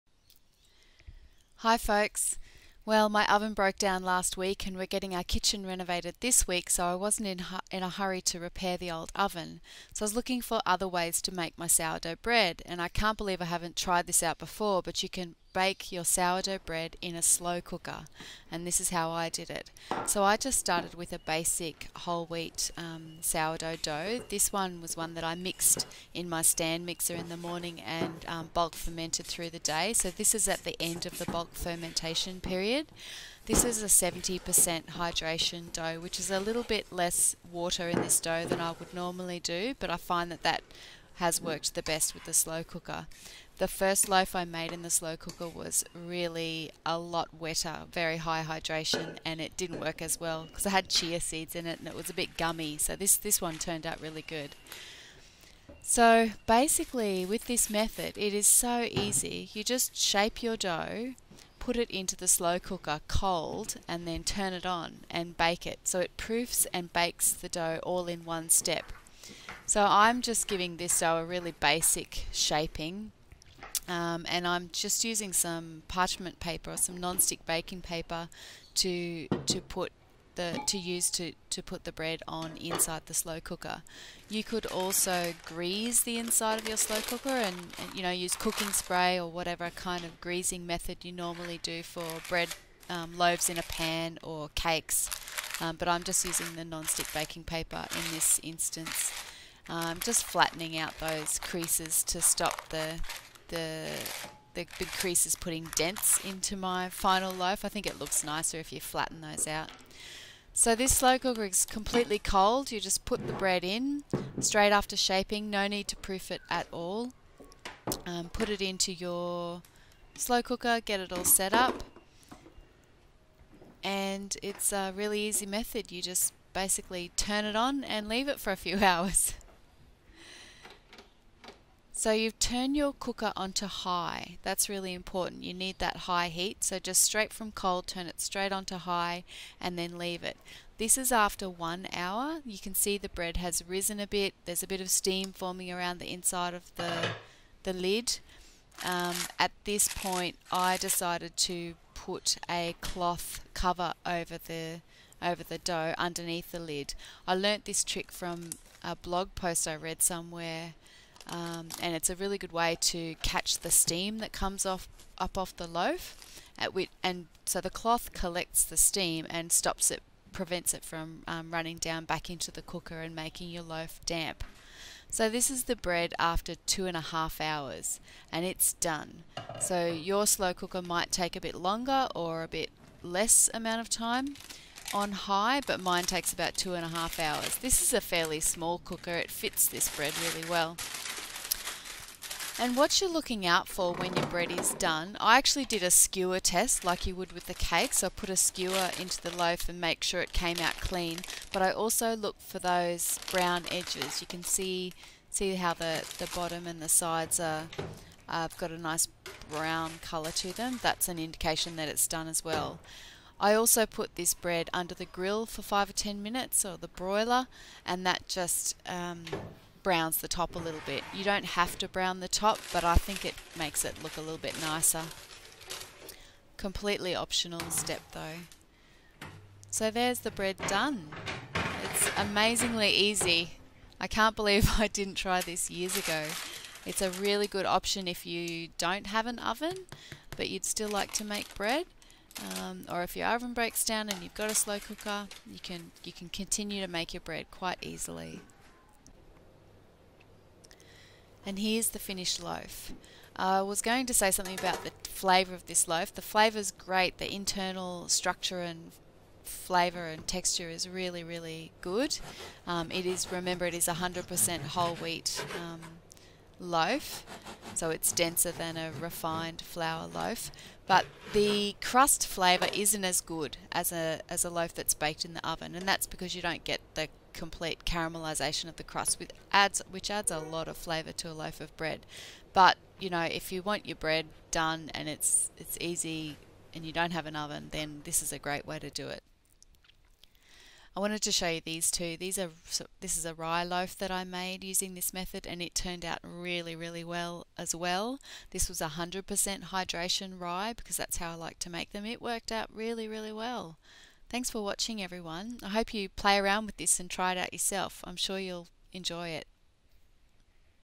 No Oven? You can still make great sourdough bread :)
Hello friends! This crockpot whole wheat / whole grain sourdough bread was such a revelation to me, I just had to share it! The loaf in this video was only the second I ever made with this method, but I thought it would turn out well so I took the chance and made a recording. I'm glad I did!

Here's the recipe. You can use my recipe, or feel free to try your own. Just don't use a very wet dough, it may come out gummy.

500g whole wheat flour (I used freshly milled flour however you can use store-bought)
350g water
1.5 tsp salt
2/3 cup whole wheat sourdough starter

Mix the water and flour together and allow the dough to rest (autolyse) for 30-60 minutes. Then mix in the salt and starter and knead the dough in a mixer if you have one, or knead by hand until the dough becomes smooth. You can also make this bread 'no knead' style, or do stretch and folds throughout the bulk fermentation. It will work either way.

Ferment the dough until it has doubled in size and is visibly bubbly. This could take anywhere between 4 and 10 hours, depending on your temperature.

Once the dough is fermented, shape it into a round loaf shape and place into your slow cooker. Use non-stick baking paper or grease the slow cooker pot as you would for a pan loaf. Note that if you have a very large slow cooker you can also put your bread into a pan and place the pan inside the slow cooker. I haven't tried this method (my cooker is small) but I've heard that it works.

Cook your bread on high for 2-3 hours. The time your loaf takes depends on your cooker and the size of the loaf. Look out for browning on the bottom and sides. You can also do a skewer test (as you would for a cake) to check that the bread is done. If the skewer comes out clean, your bread is cooked!

Cool and enjoy the bread as usual.

Thank you for your ongoing contributions, comments, feedback and support.